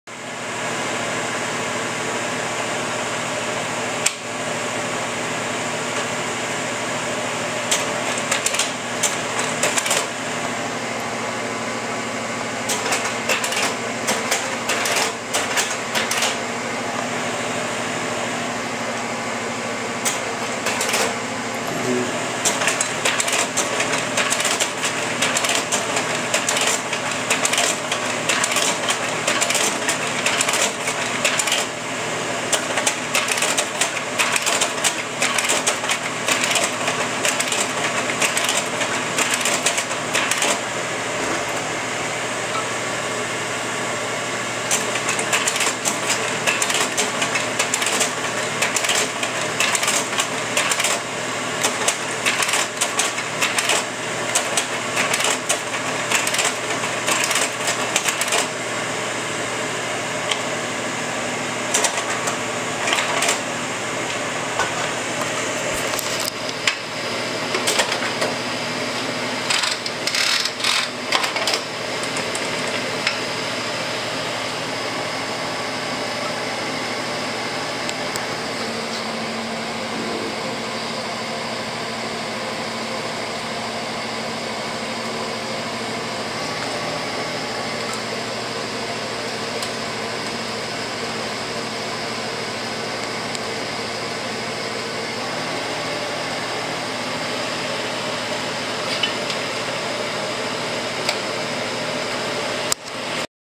Kodak Carousel 4400 Projector demo
Demonstration of Kodak Carousel 4400 Projector functioning for sale on ebay.